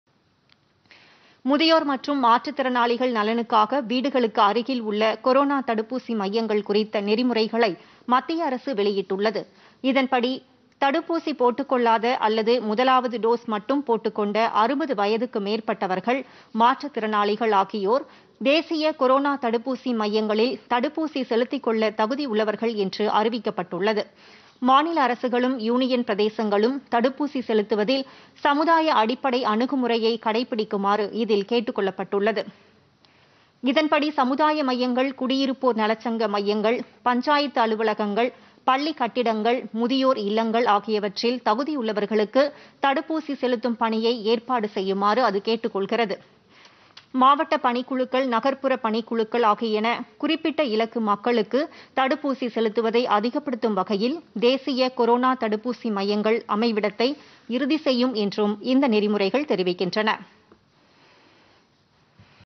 முதியோர், மாற்றுத்திறனாளி தடுப்பூசி வழிமுறைகள் வெளியீடு
முதியோர் மற்றும் மாற்றுத் திறனாளிகள் நலனுக்காக வீடுகளுக்கு அருகில் உள்ள கொரோனா தடுப்பூசி மையங்கள் குறித்த நெறிமுறைகளை மத்திய அரசு வெளியிட்டுள்ளது.
  இதன்படி, தடுப்பூசி ‍போட்டுக் கொள்ளாத  அல்லது முதலாவது டோஸ் மட்டும் போட்டுக் கொண்ட 60  வயதுக்கு மேற்பட்டவர்கள்,   மாற்றுத்திறனாளிகள் ஆகியோர் தேசிய கொரோனா தடுப்பூசி மையங்களில் தடுப்பூசி செலுத்திக் கொள்ள தகுதி உள்ளவர்கள் என்று அறிவிக்கப்பட்டுள்ளது.